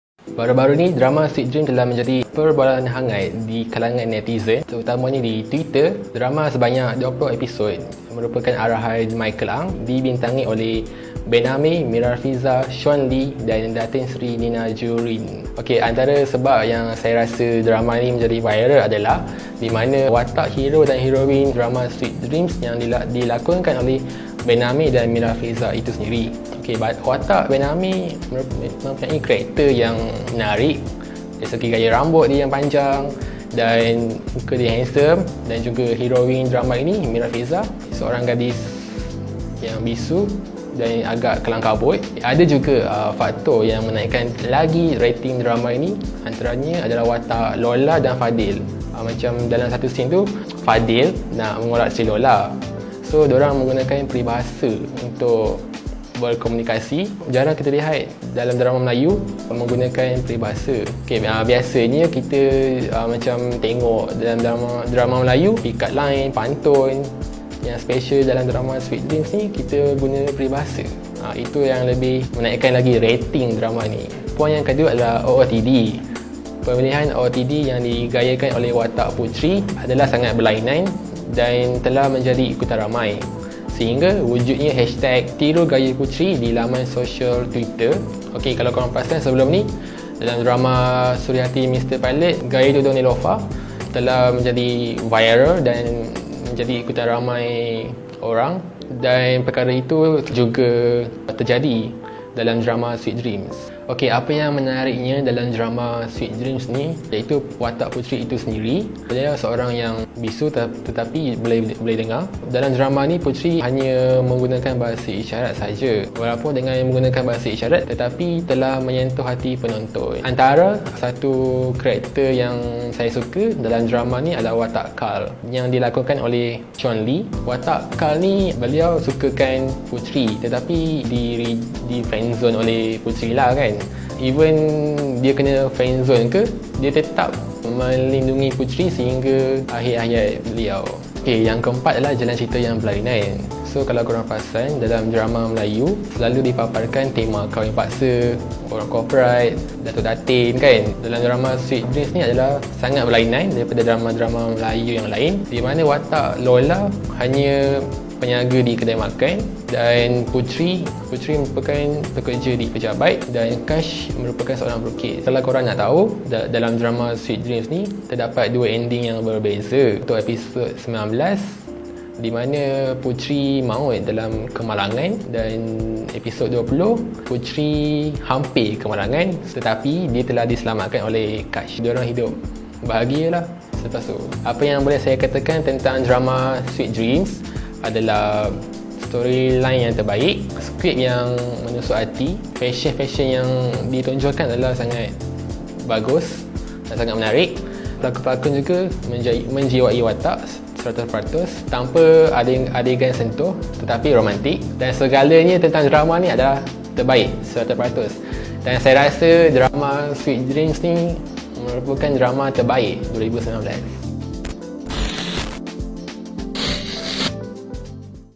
5 SEBAB DRAMA SWEET DREAMS BEST | DRAMA REVIEW | IQMALFIRDAUS X BORAKDAILY
5 sebab DramaSweetDreams best. Demam drama Sweet Dreams memang menjadi2. Dah tamat pun, peminat memang tak boleh move on dengan DramaSweetDreams ni! Nak tahu sebab apa? Dengar penjelasan abang @iqmal.firdaus ni! Thank you BorakDaily ❤️🌹

Thanks 😊